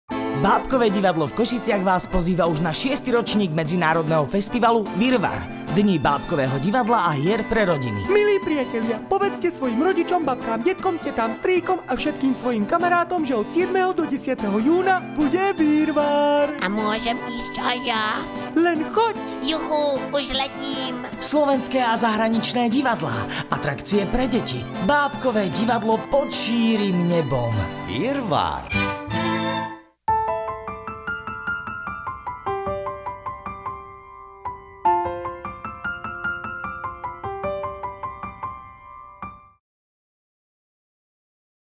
Festival Virvar - Dni bábkového divadla a hier pre rodiny
Pozývame Vás na 6. ročník festivalu Virvar, ktorý sa uskutoční v Košiciach v dňoch 7. - 10. júna 2018. Viac informácií nájdete na